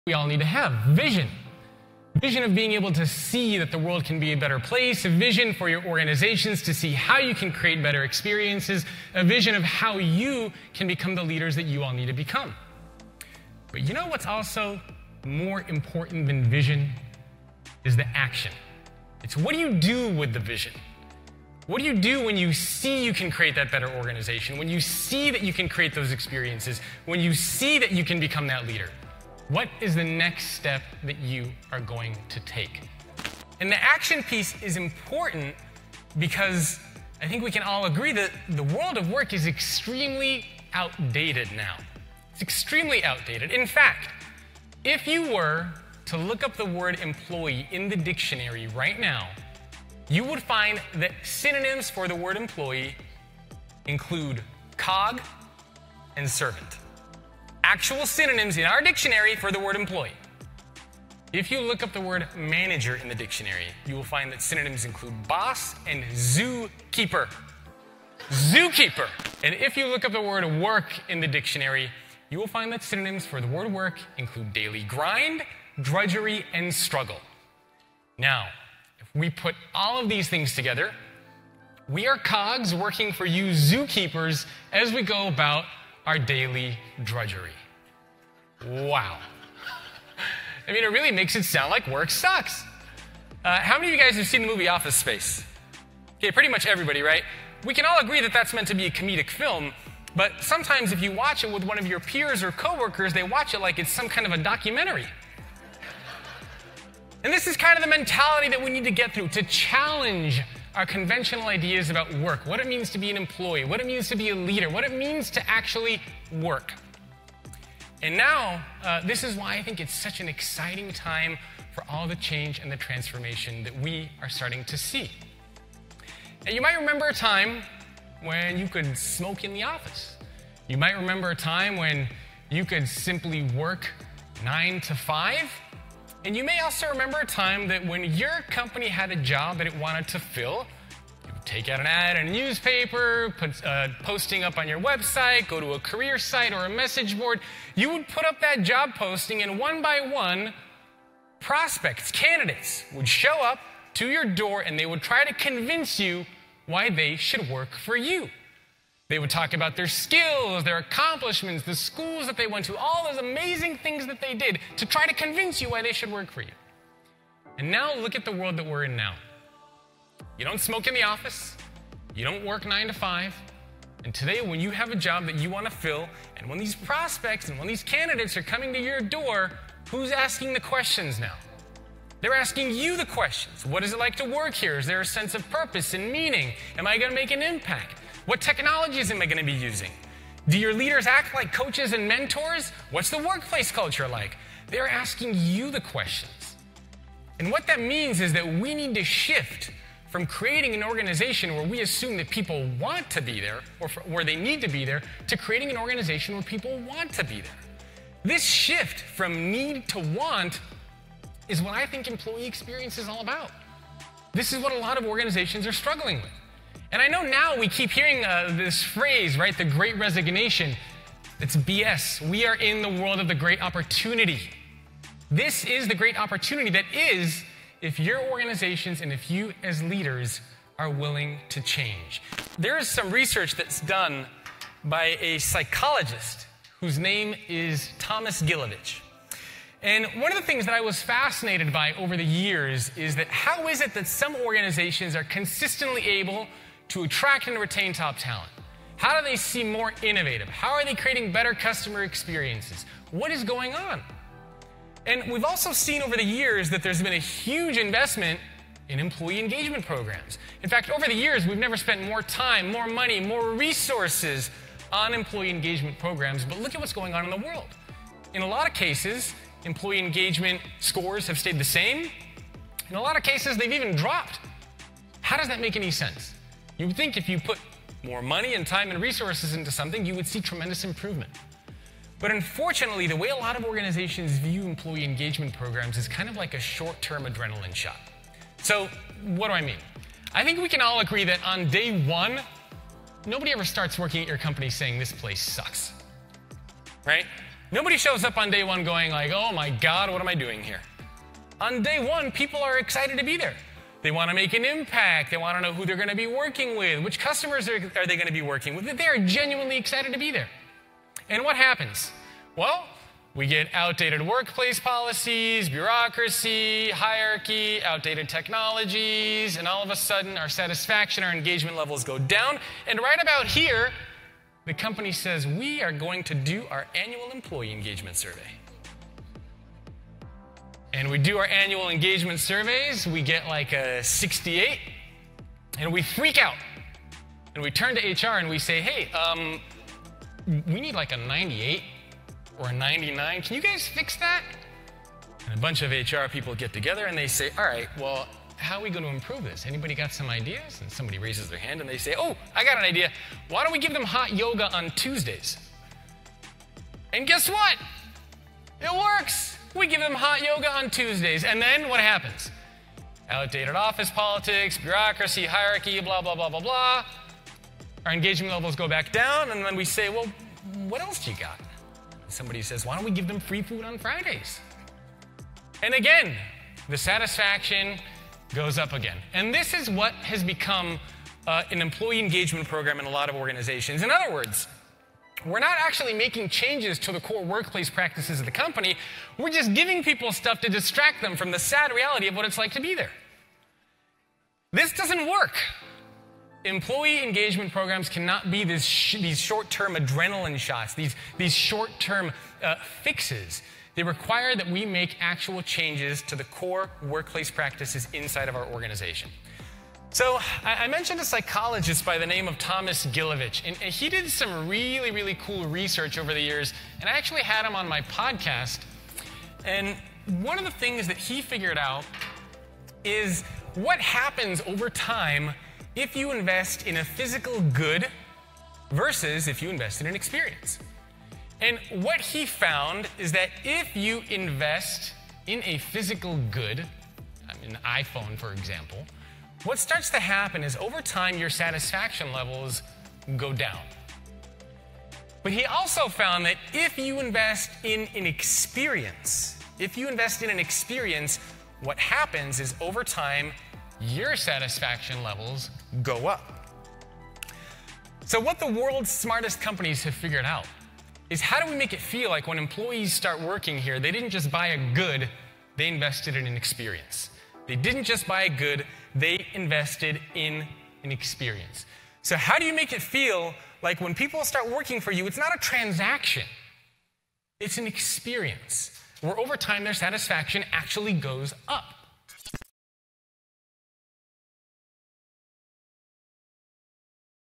Why the Future of Work is Extremely Outdated | Jacob Morgan
We can all agree that the world of work is extremely outdated.

If you were to look up the word employee in the dictionary, you would find synonyms like cog and servant.

If you look up the word manager in the dictionary, its synonyms include boss and zookeeper. And if you look up the word work in the dictionary, you'll find synonyms like daily grind, drudgery, and struggle.

This is the mentality we need to get through to challenge our conventional ideas about work, what it means to be an employee, and what it means to be a leader.

We all need to have vision. A vision of being able to see that the world can be a better place. A vision for your organizations to see how you can create better experiences. A vision of how you can become the leader you need to become.

But, what's more important than vision is the action. It's what you do with the vision.

When you see that you can create a better organization, you can create great experiences.

When you see that, you can become that.